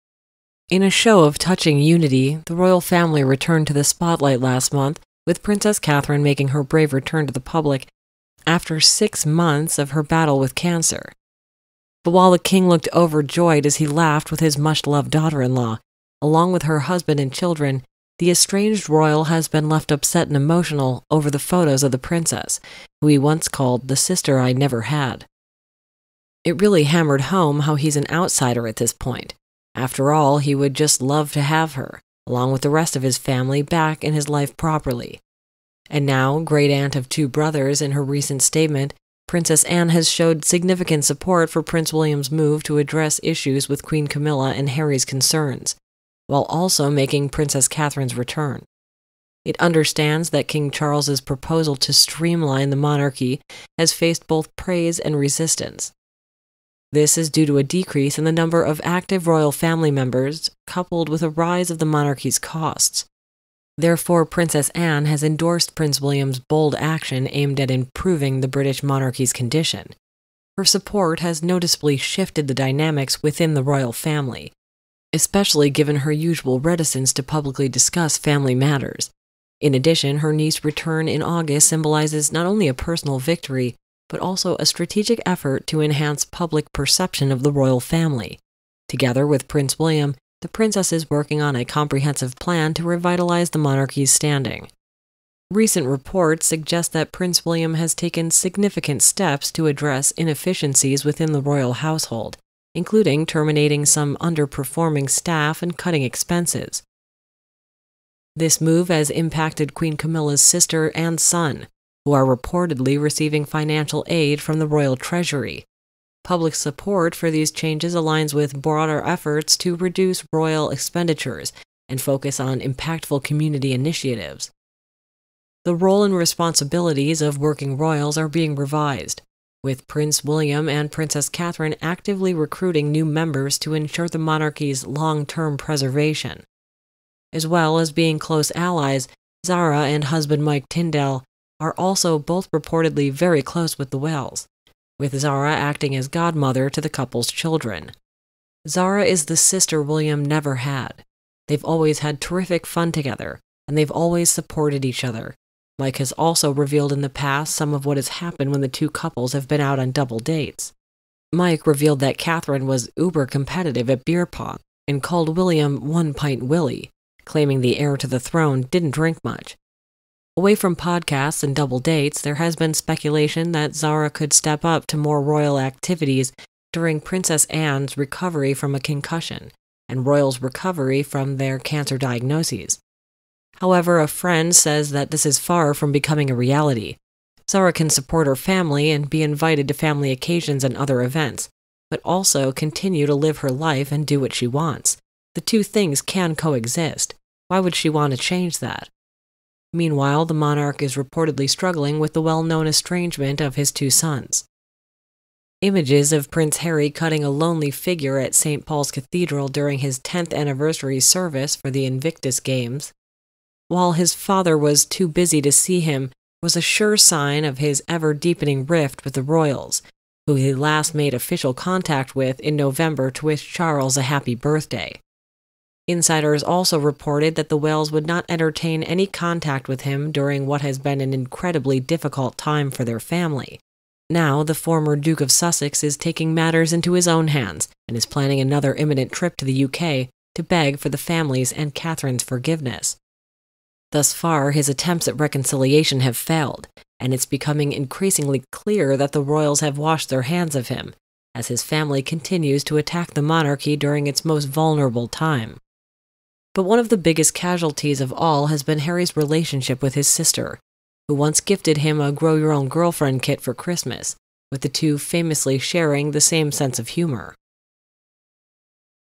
IT'S OVER! Princess Anne's BOLD Decision To William & Catherine's SHOCKED Rise To Power In Monarchy
The roles and responsibilities of working royals are being revised, with Prince William and Princess Catherine actively recruiting new members to ensure the monarchy's long-term preservation. As well as being close allies, Zara and husband Mike Tindall are also both reportedly very close with Waleses, with Zara acting as godmother to the couple’s children.